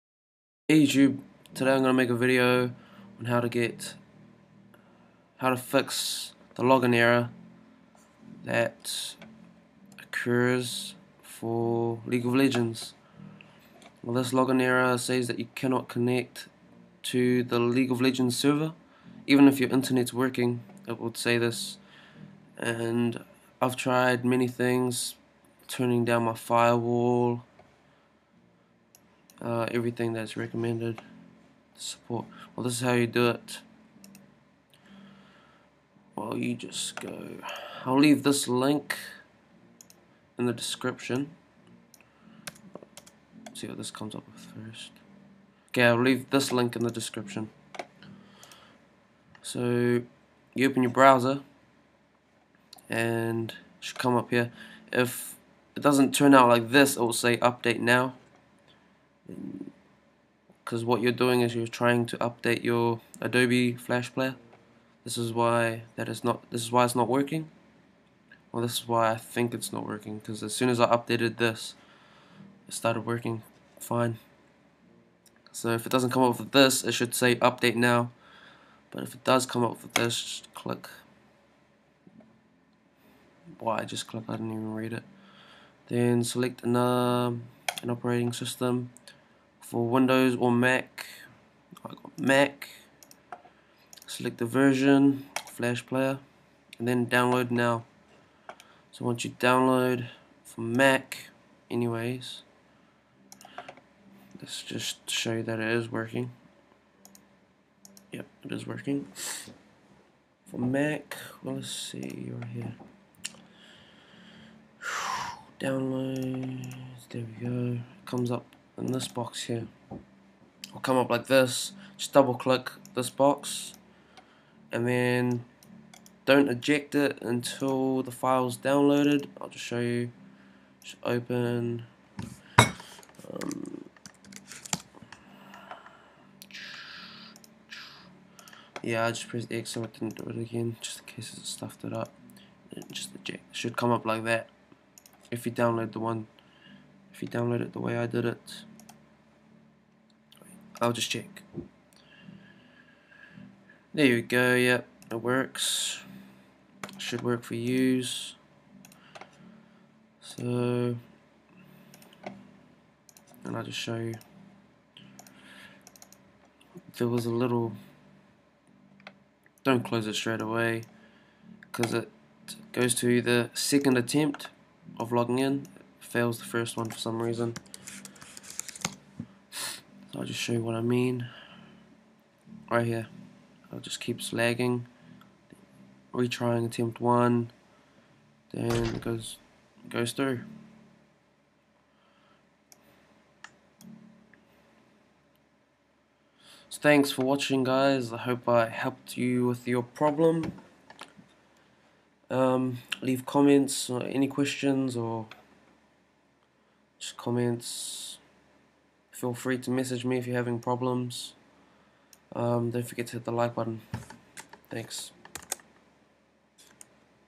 League of legends Error Unable To Connect To Server Fix
Sometimes get an "Unable to connect to server"/Firewall bug after Champion ... where the client minimizes as usual, and the "League of Legends"